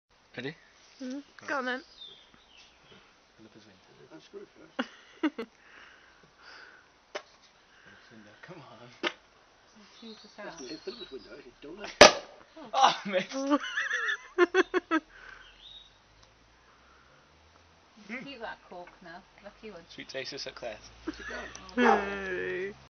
GCSE Celebration Champagne
My own gcse celebration bbq's champagne cork being popped!